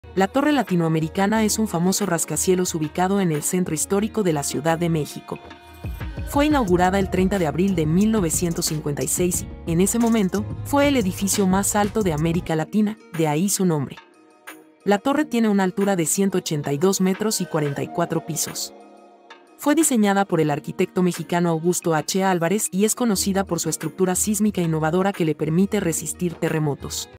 ¿Que es la Torre Latinoamericana?
La Torre Latinoamericana es un famoso rascacielos ubicado en el centro histórico de la Ciudad de México. Fue inaugurada el 30 de abril de 1956 y, en ese momento, fue el edificio más alto de América Latina, de ahí su nombre. La torre tiene una altura de 182 metros y 44 pisos. Fue diseñada por el arquitecto mexicano Augusto H. Álvarez y es conocida por su estructura sísmica innovadora que le permite resistir terremotos.